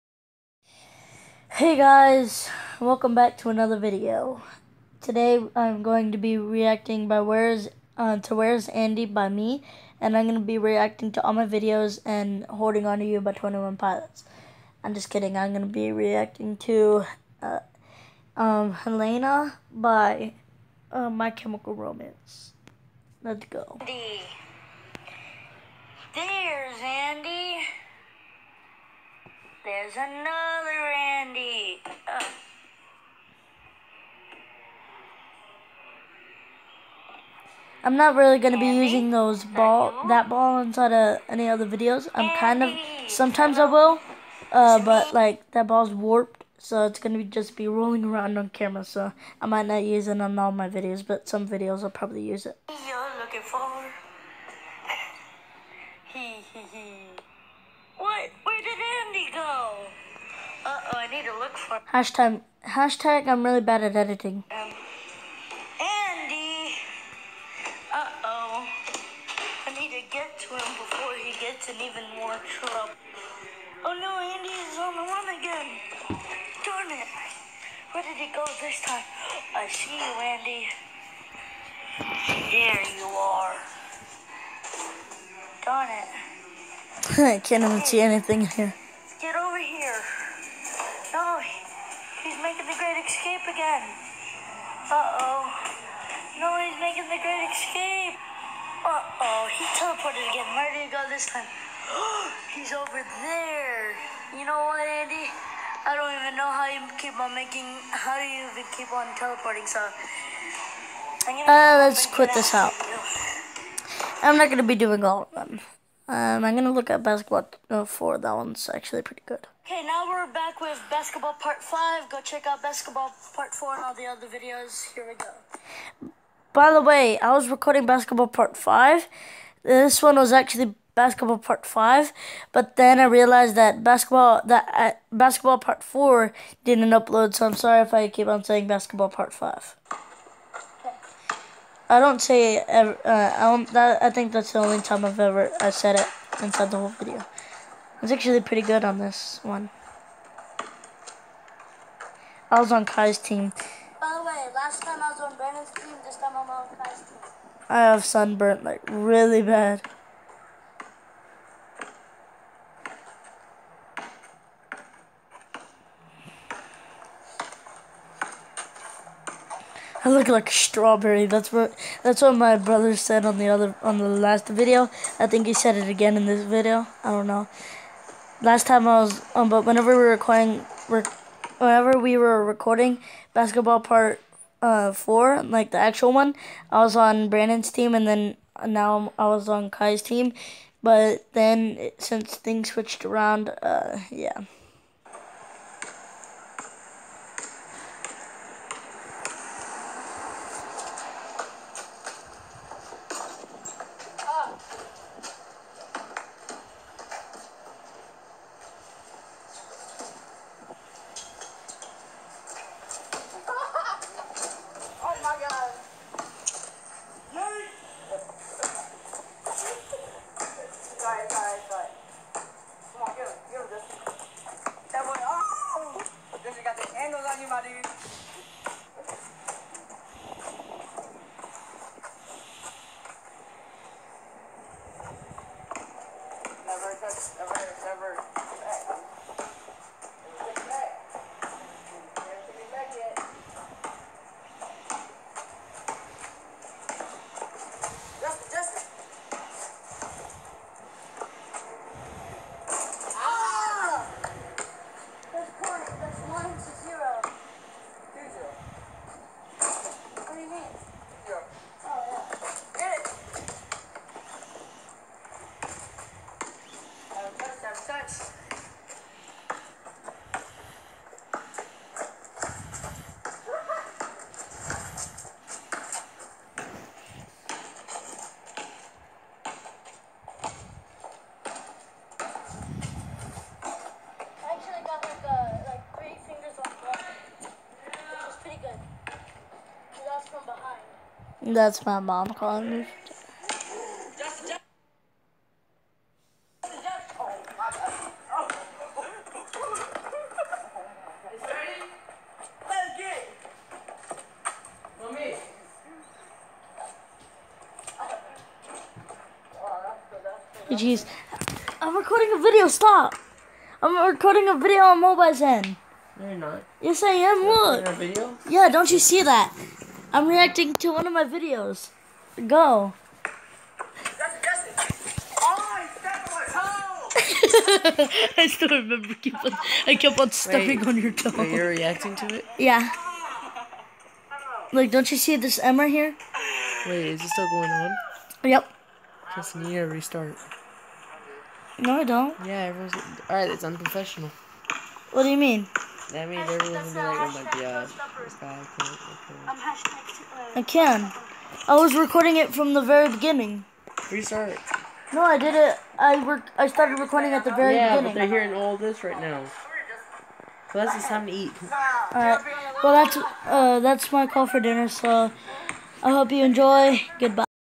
Reacting to two of my vids!!
Today I'm reacting to basketball 4 and a little bit of where's Andy? Enjoy (sorry for all the shouting/talking in the back by my mum and brother at the end)